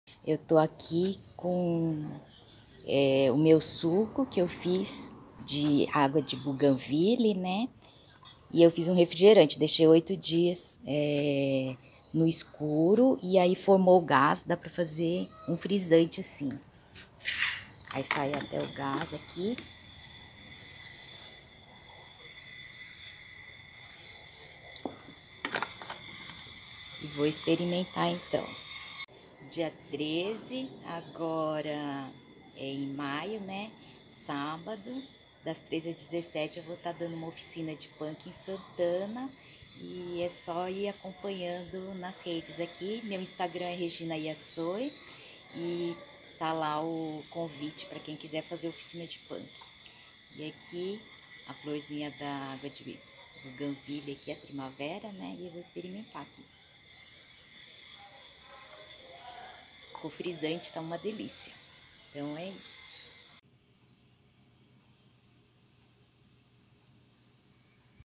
refrigerante natural feito com flor de primavera, água de bougainvillea PANC
Receita do frisante do CHÁ-SUCO ÁGUA DE BOUGAINVILLEA, da flor da primavera

faça o chá das flores, para cada litro de água fervente, coloque cerca de 30 flores de primavera, retire os cabinhos e florezinhas miúdas do miolo e tampe, ferva mais uns 2 min. Espere esfriar, coe e adoce. Coloque 1 limão espremido, envaze em garrafa pet, deixando uma parte vazia e aperte a garrafa para tirar o máximo de ar;  tampe e deixe de 7 a 8* dias no escuro para formar gás. Depois leve para refrigerar e consuma gelado.

*observação, se estiver muuuito quente, veja se em 5 ou 6 dias a garrafa estufou, se sim, é sinal que já gaseificou e pode gelar e consumir. Pode colocar uma florzinha de primavera no copo e gelo para servir